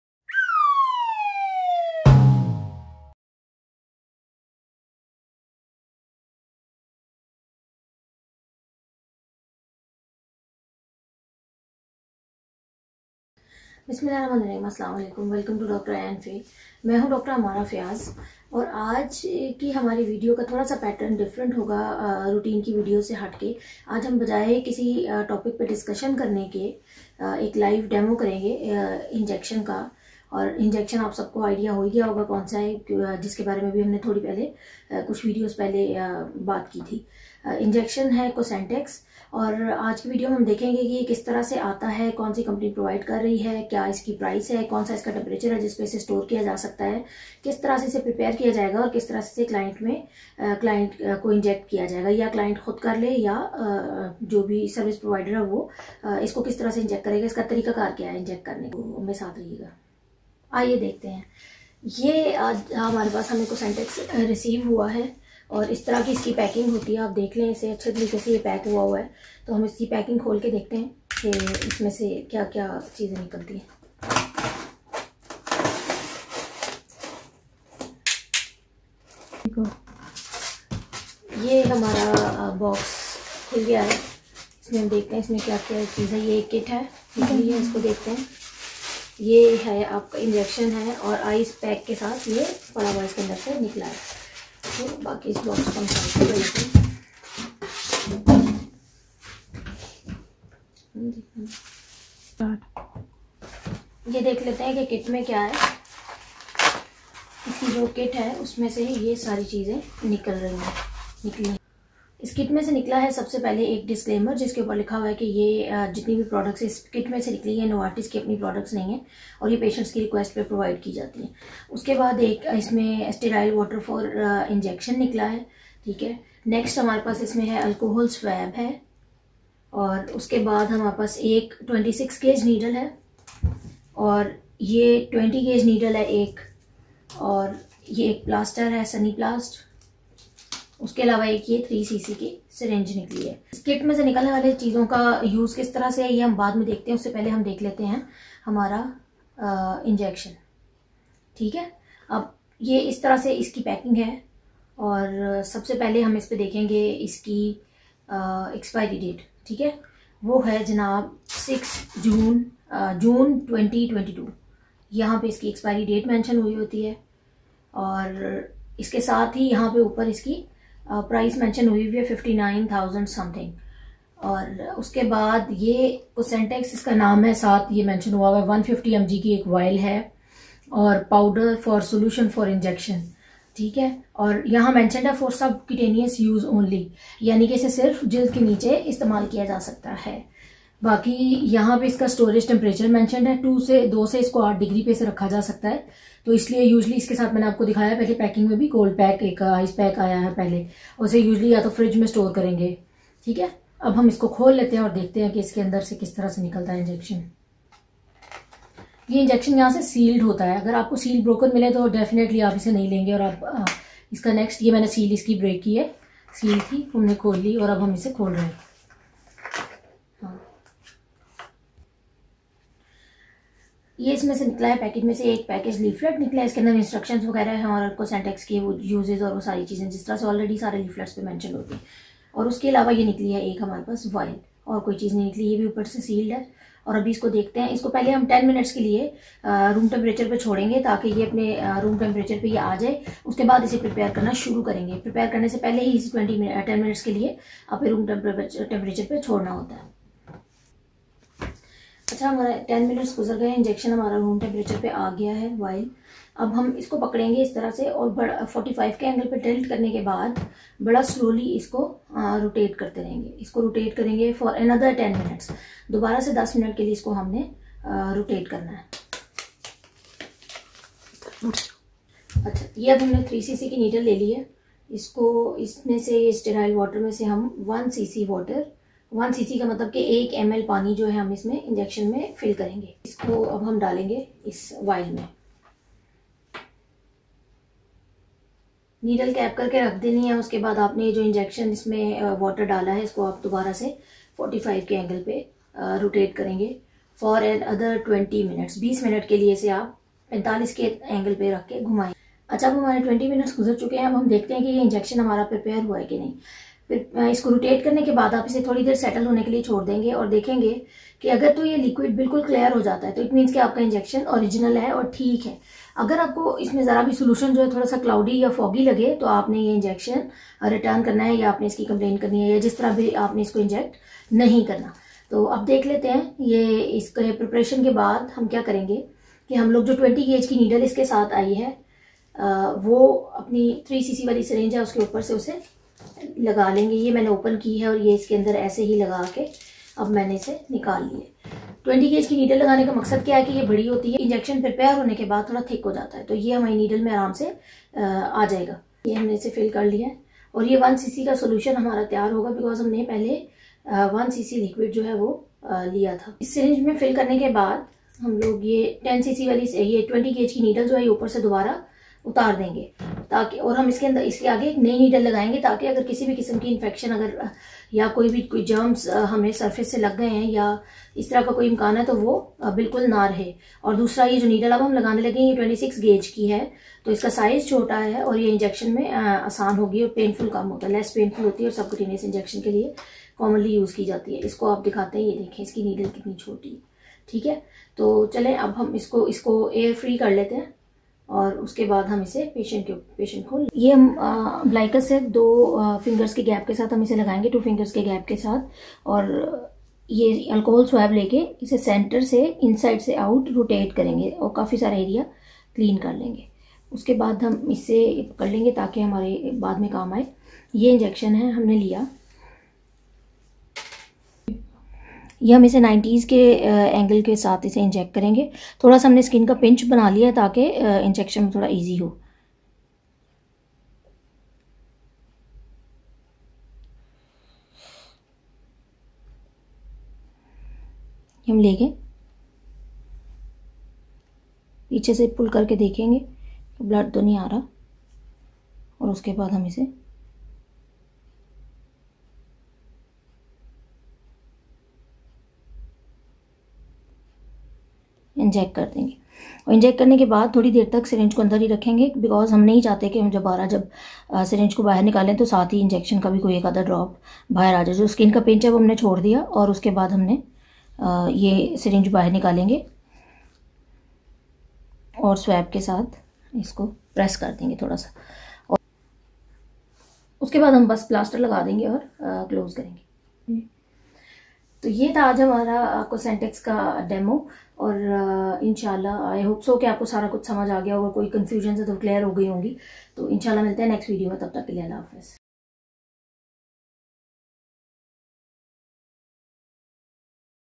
Injection Cosentyx |Price |How to Inject cosentyx | Dr Ain Fay | Live Demo |video # 14
Injection Cosentyx | How to Inject cosentyx |Urdu | Dr Ain Fay | video # 14